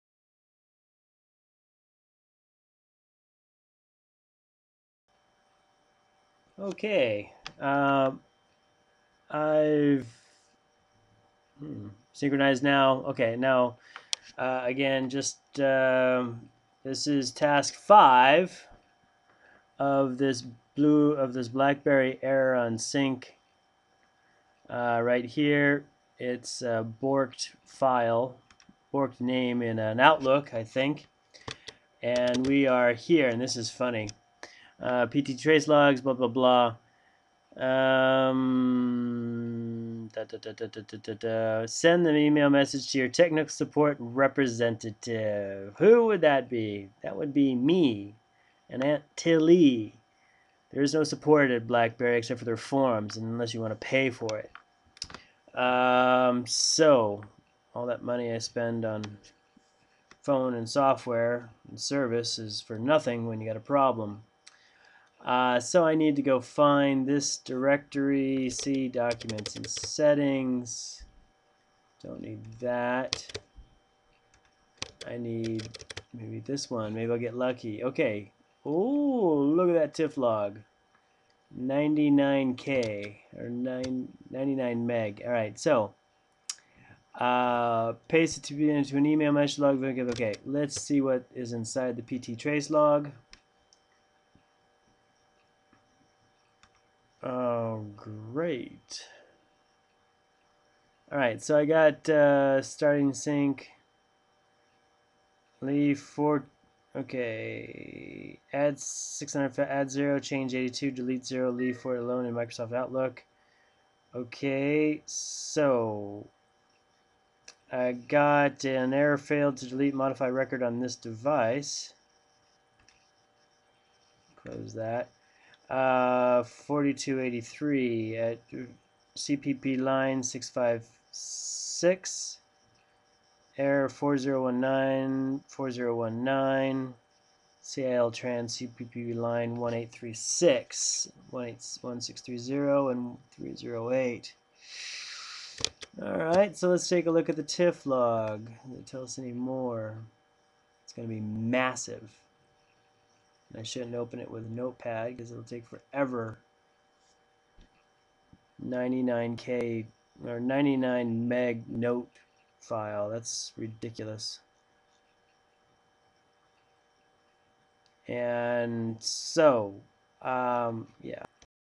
Blackberry Error code 0x80040fb3 task 5 of six
99M text file full of error data for me to sort through
Blackberry Error code - 0x80040fb3